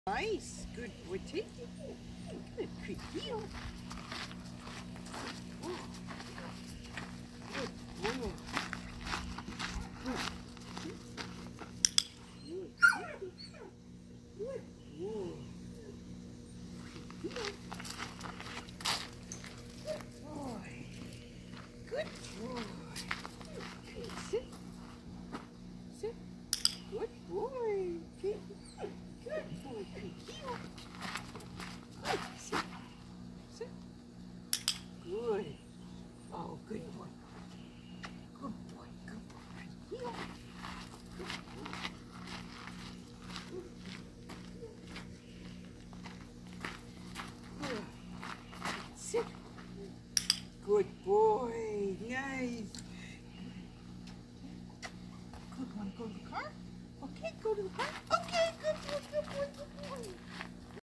CREDE CGCA & CGCU preparations
Crede is doing a good job heeling!  He works with joy and with enthusiasm :-)